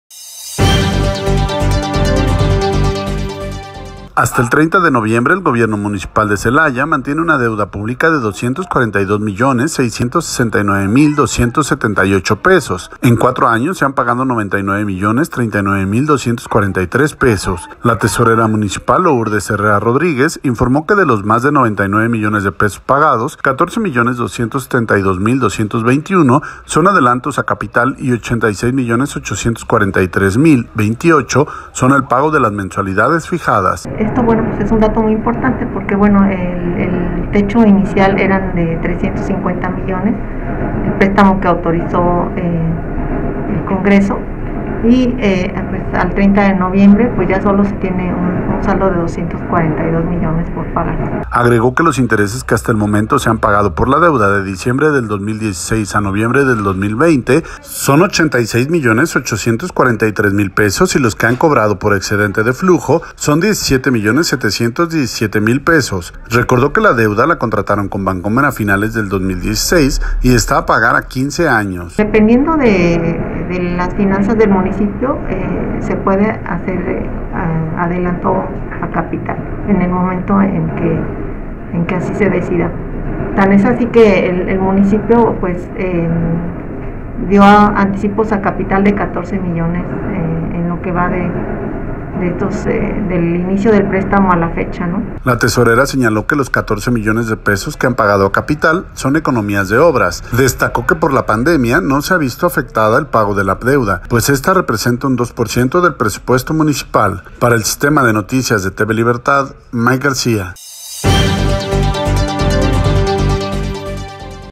Administración de Celaya adeuda 242 millones de pesos a Bancomer
El Gobierno Municipal de Celaya Guanajuato, adeuda 242 millones de pesos a Bancomer solicitados en la administración anterior encabezada por Ramón Lemus Muñoz Ledo